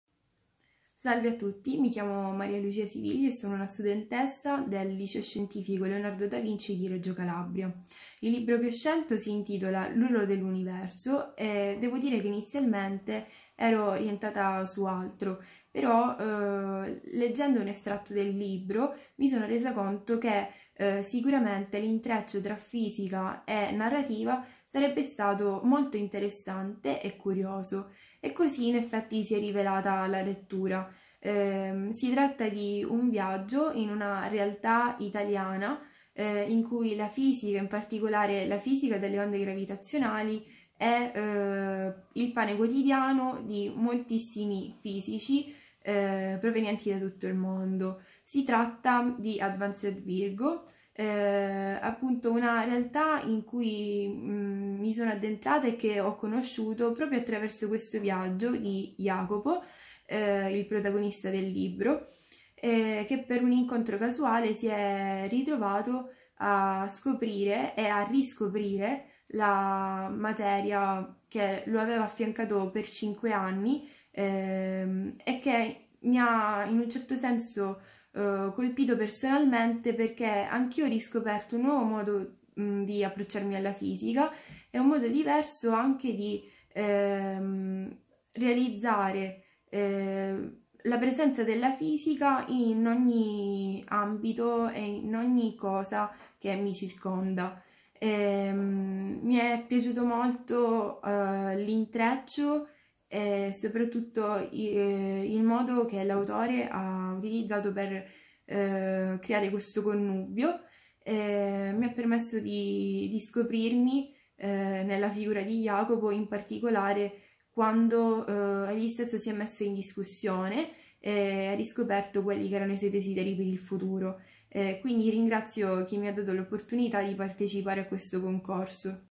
Premio Asimov 2020: Marialucia Siviglia, Regione Calabria
Marialucia Siviglia, studentessa del Liceo Scientifico "Leonardo da Vinci" di Reggio Calabria, è autrice di una delle recensioni vincitrici del premio Asimov 2020 per la regione Calabria.

Il Premio Asimov è un riconoscimento riservato ad opere di divulgazione e di saggistica scientifica particolarmente meritevoli. Esso vede come protagonisti sia gli autori delle opere in lizza che migliaia di studenti italiani, che decretano il vincitore con i loro voti e con le loro recensioni, a loro volta valutate e premiate.